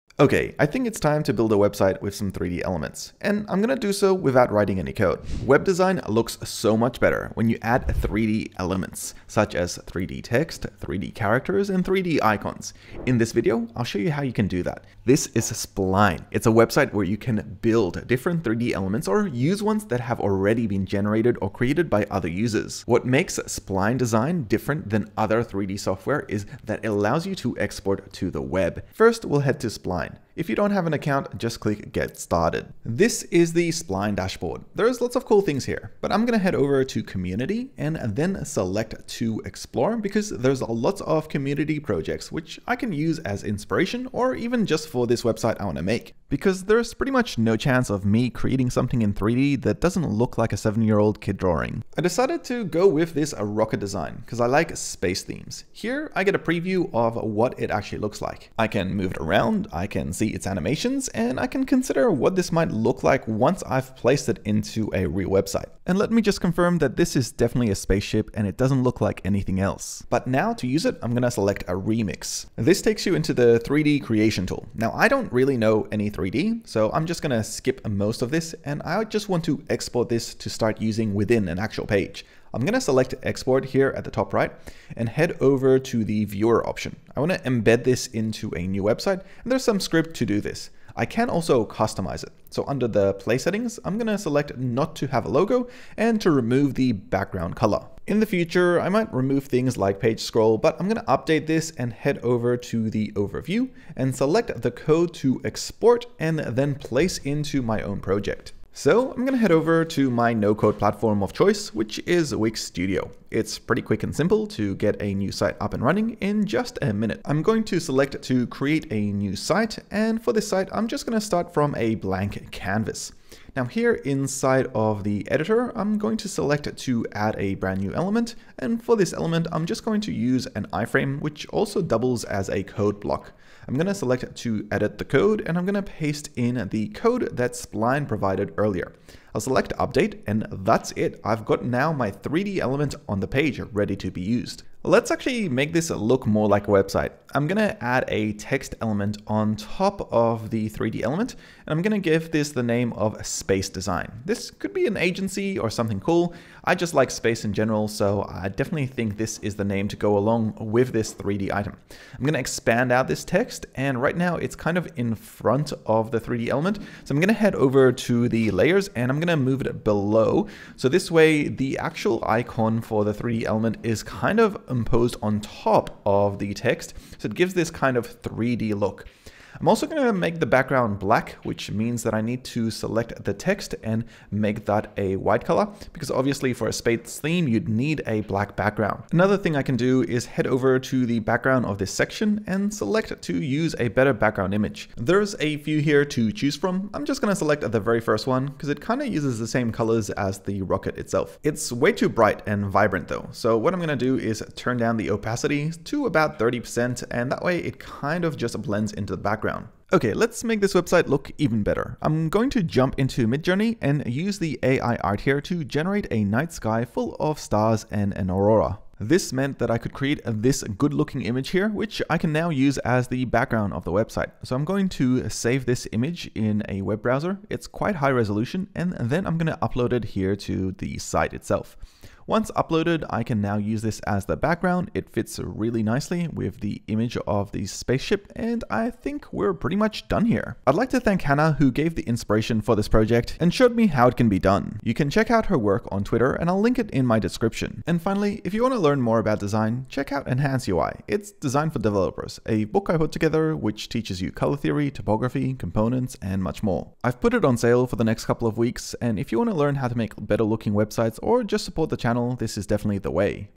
How to create 3D Website Designs With No Code
Learn how to add 3d elements to your web designs. These can be interactive 3d objects that you can place within your user interface to bring more life to a site design. This video I'll add elements without even using iFrames, but instead using spline and it's code blocks to embed 3d objects that can be moved by your mouse, or scroll or anything!

Software & Discounts!